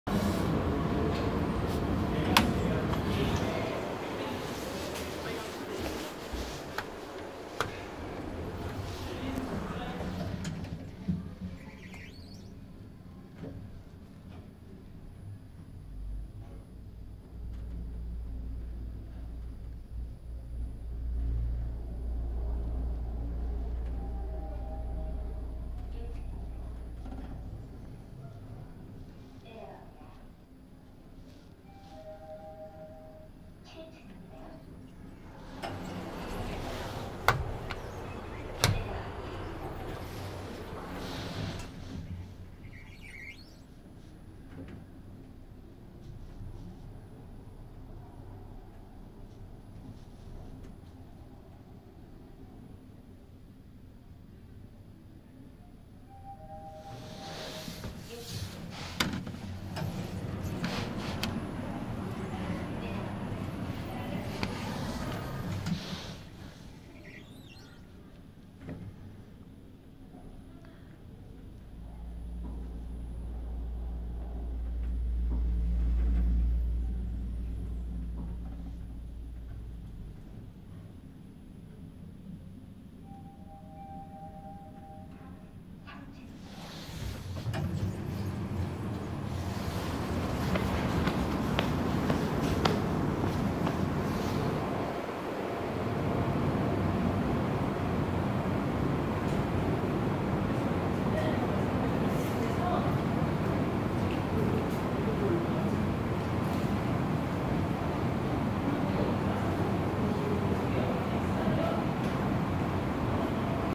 서울특별시 용산구 한강대로23길 55 (한강로3가) 구.현대아이파크몰(현.HDC아이파크몰) 나머지구간 26 현대엘리베이터 탑사기
해당 구간은 화질 개선을 위한 2차 탑사분이며, 보호막은 아직 제거되지 않았습니다.